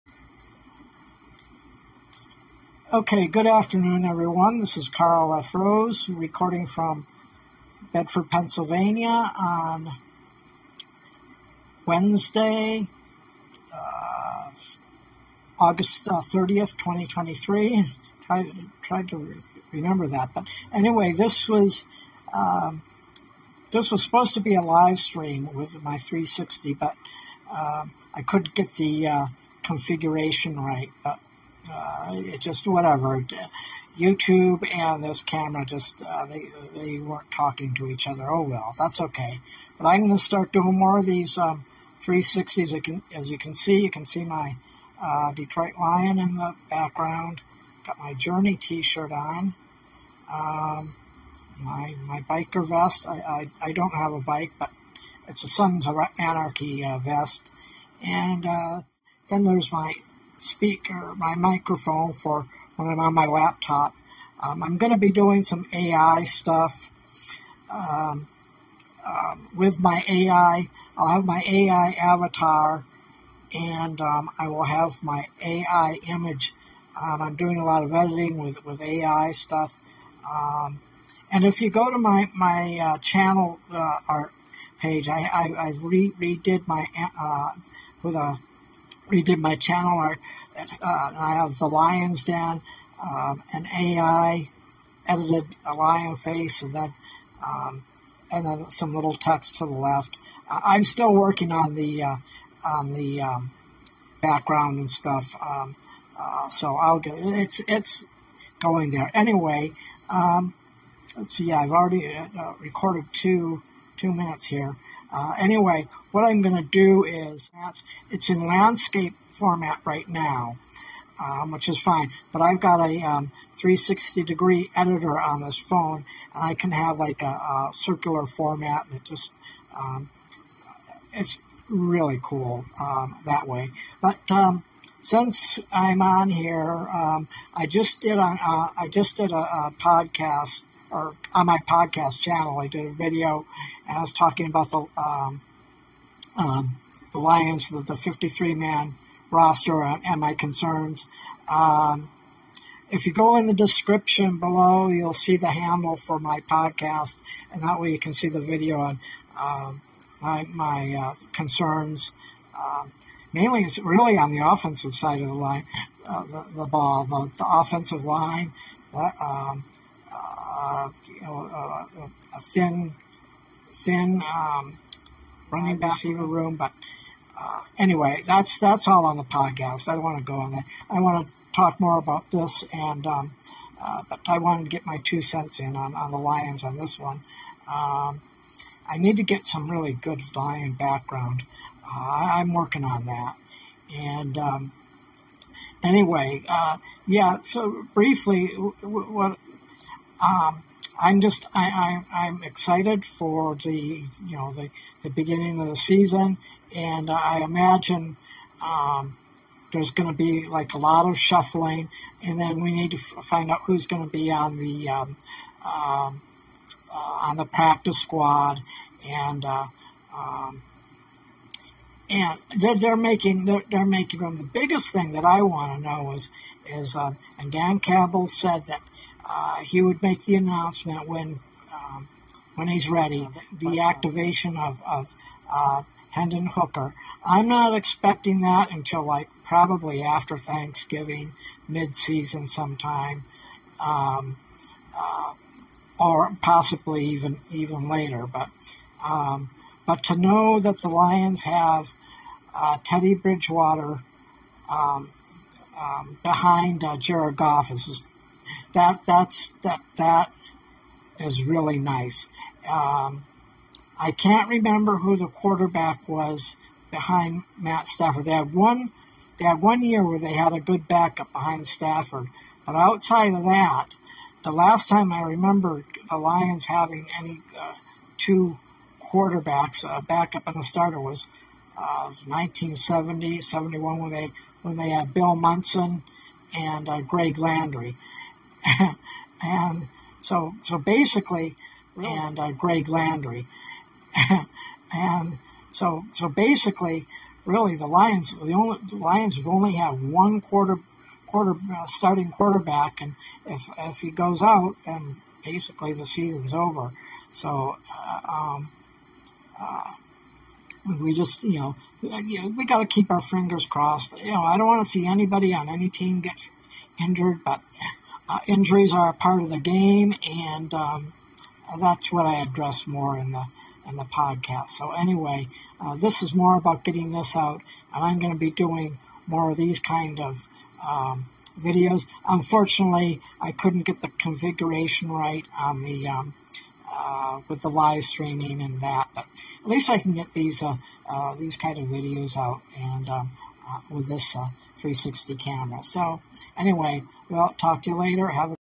The full video of my new 360 degree style set up plus some thoughts on the Lions roster
Here is the update video on my new 360 degree style format edited to a circular format. Yesterday I posted the landscape version. I also speak a little bit about the Detroit Lions.